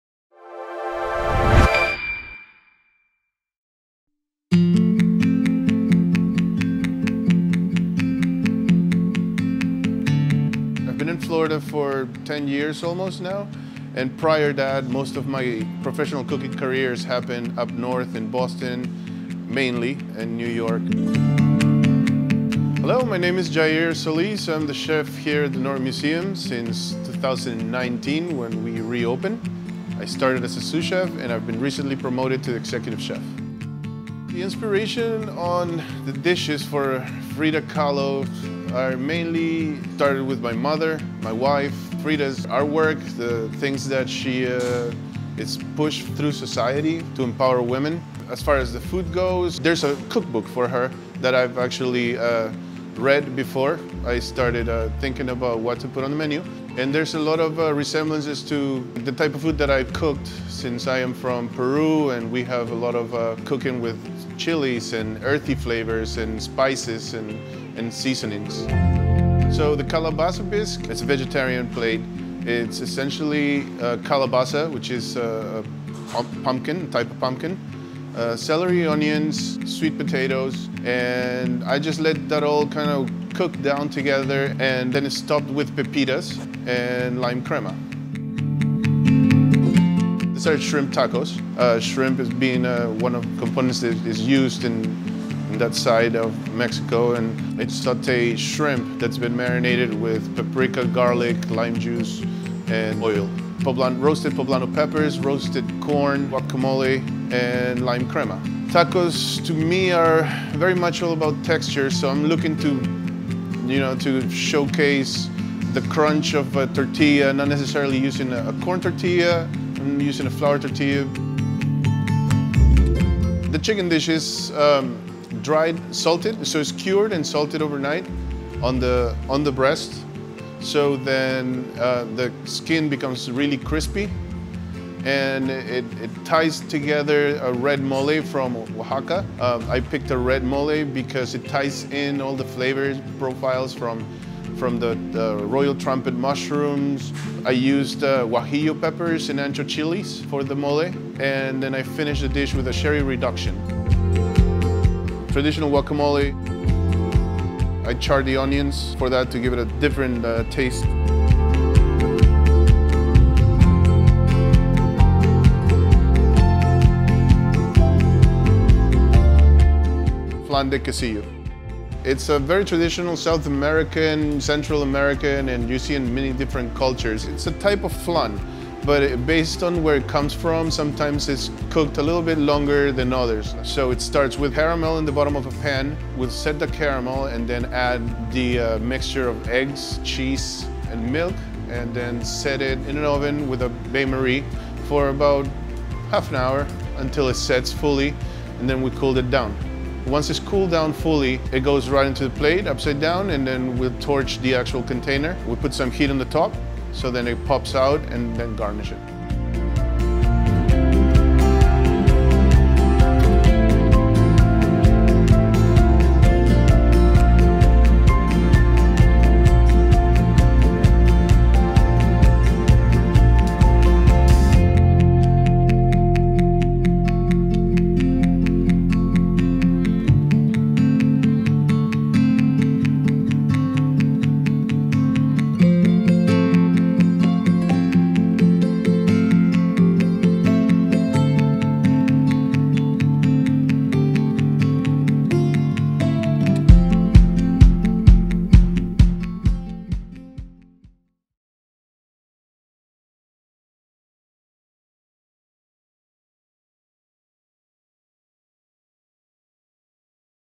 Culinary Creations | Frida Kahlo at The Restaurant
Executive Chef Jair Solis introduces us to his new Frida Kahlo inspired menu items as part of The Norton's upcoming Frida Kahlo, Diego Rivera and Mexican Modernism from the Jacques and Natasha Gelman Collection Exhibition.